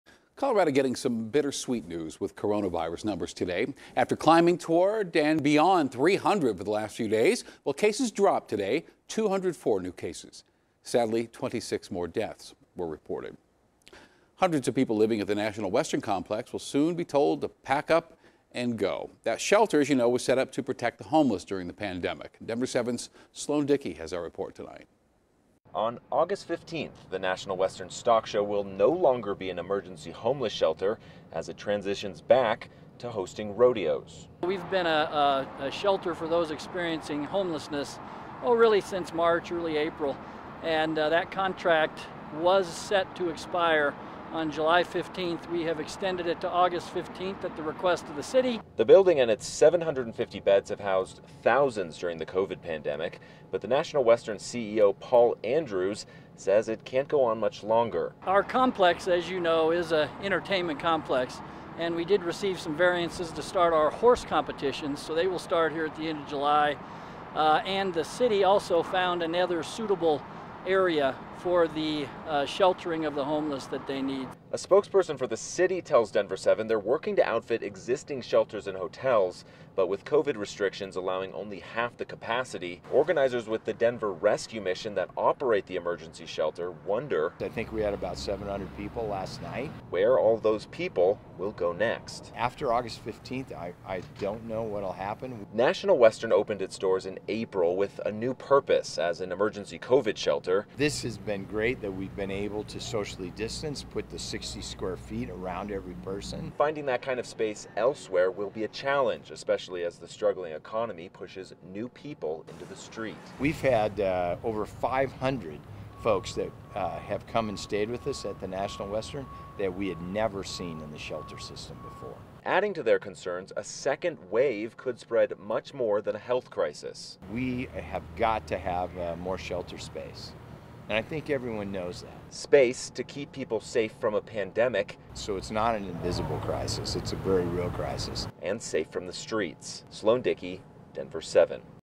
The men's emergency COVID-19 homeless shelter at National Western Complex will end on August 15
The contract between the city of Denver and the National Western Stock Show to host an emergency COVID-19 shelter is set to expire on August 15. The CEO of National Western confirmed the dates stating that the complex has been granted a variance by the state of Colorado to begin hosting equestrian competitions once again.